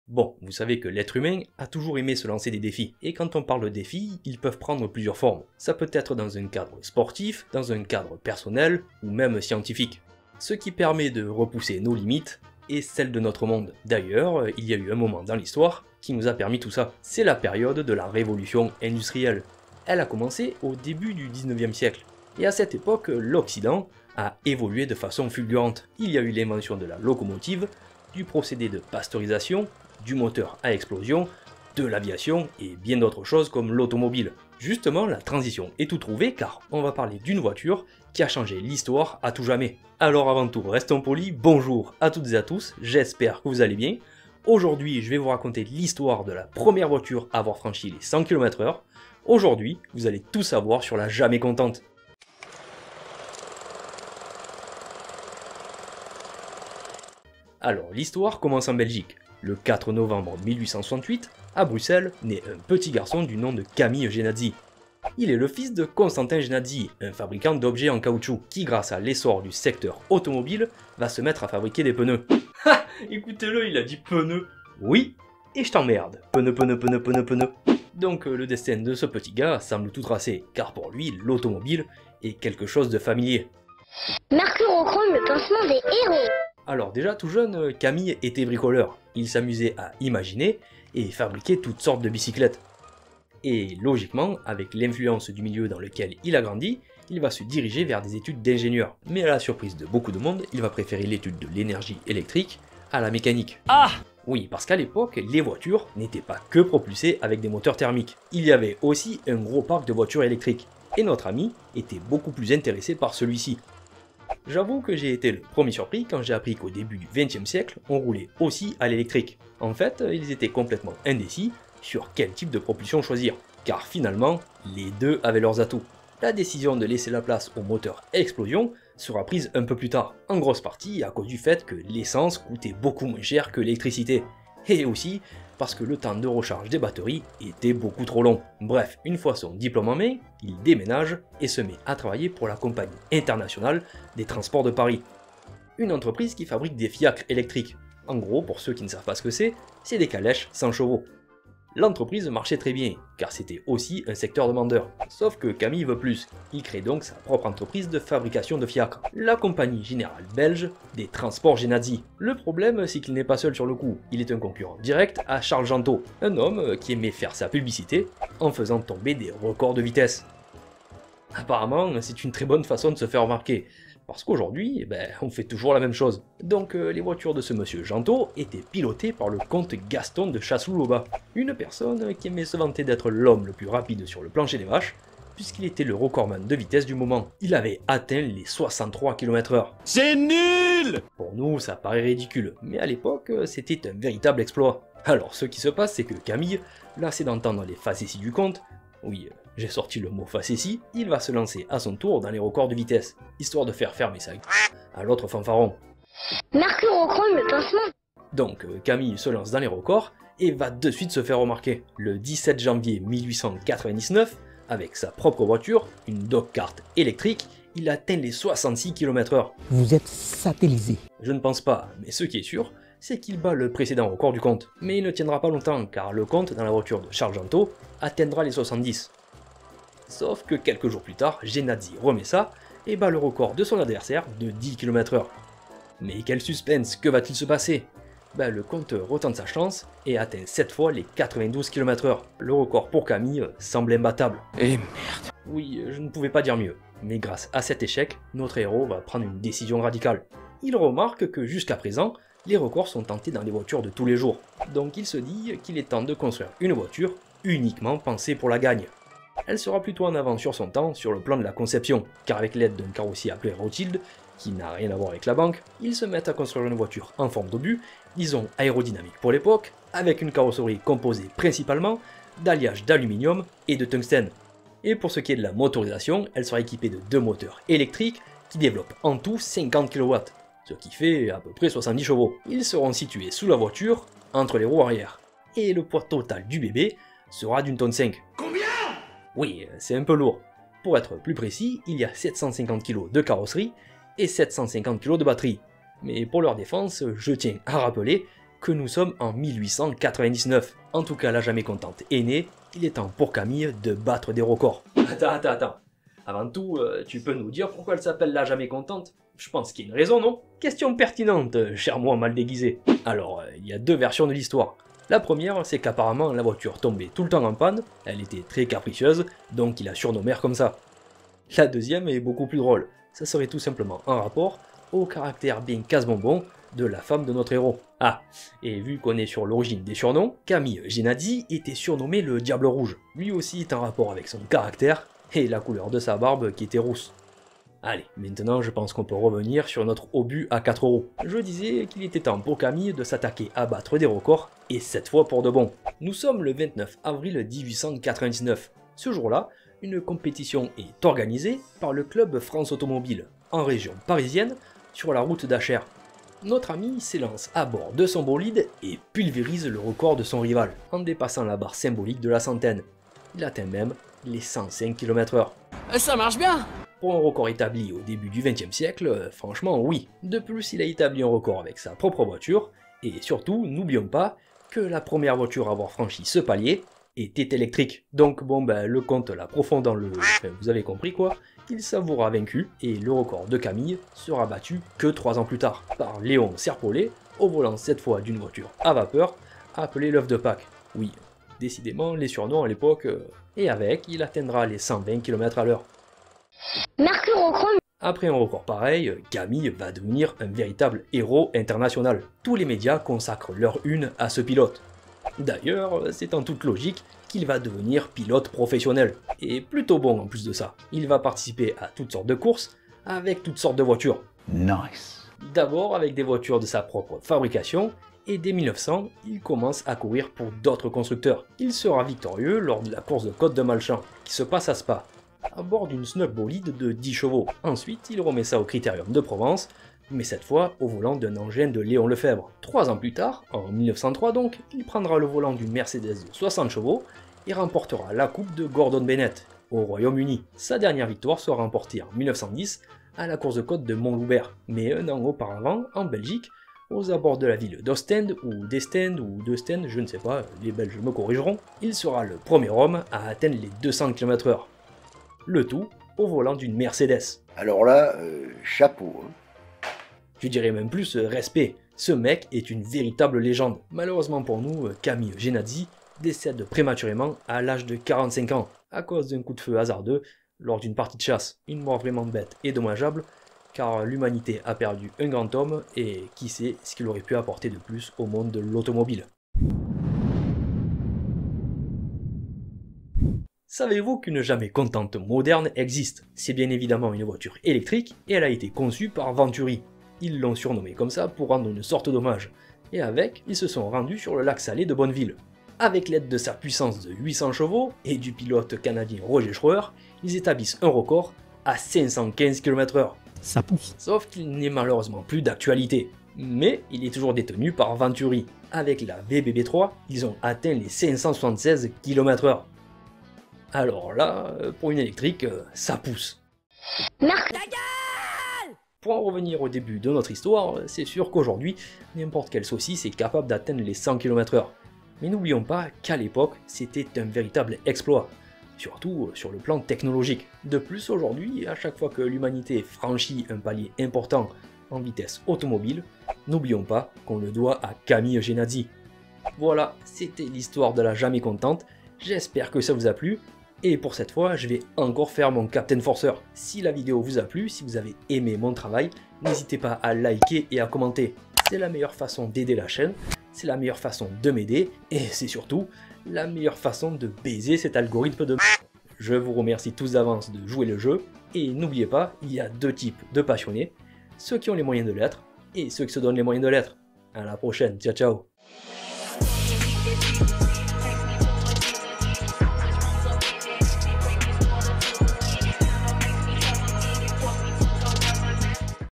LA 1ère VOITURE à avoir passé les 100 KM/H | TOUT SAVOIR sur LA JAMAIS CONTENTE et Camille Jenatzy .
Bonjour à toutes et a tous j’espère que vous allez bien , aujourd'hui , vidéo sur l'histoire de la "Jamais Contente" , la 1ere voiture à avoir franchis les 100 km/h .

Vous allez tout savoir sur son histoire et celle de son propriétaire , Camille Jenatsy , un Homme brillant qui a marqué l'histoire ;)